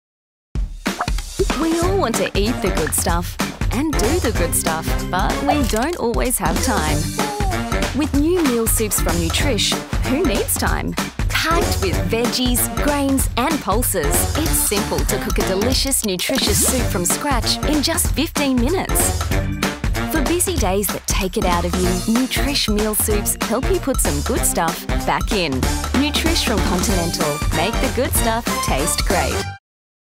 Nutrish Meal Soups TVC
When busy days take it out of you, Nutrish Meal Soup help you put some good stuff back in. Packed with veggies, grains and pulses it's the simple way to cook a delicious, nutritious soup in just 15 minutes.